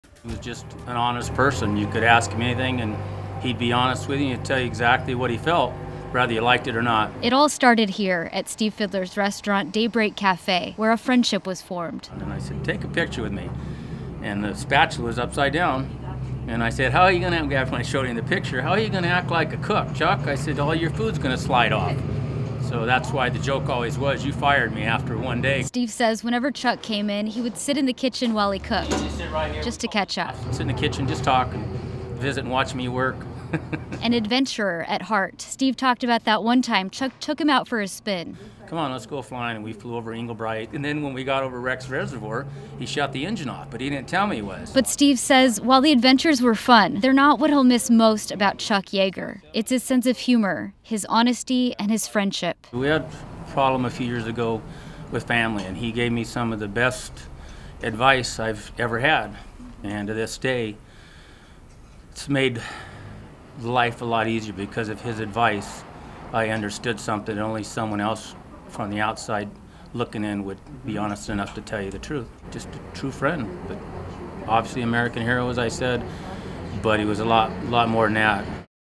'An American hero, but he was more than that' | Friend of Chuck Yaeger remembers who he was
Steve Fidler says while the adventures they went on were fun, he misses Chuck Yaeger's sense of humor, honesty and friendship more.